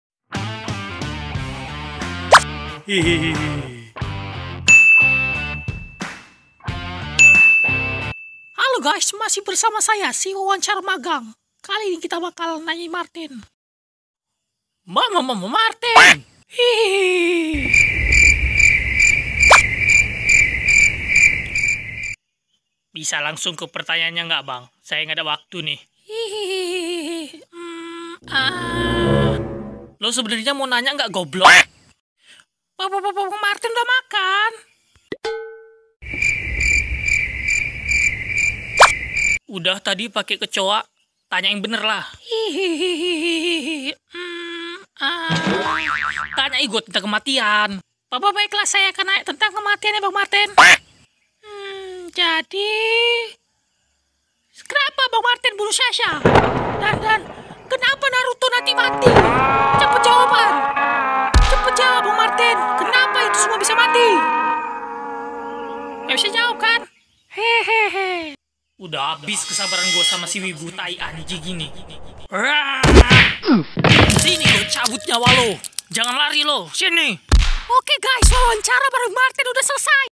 Wawancarain Martin kang cabut nyawa -kartun lucu
wawancarain Martin kang cabut nyawa
mirip kek podcast bareng Marten dan bapak muka monyet tapi ini Marten
ma ma ma Marten
yuk gass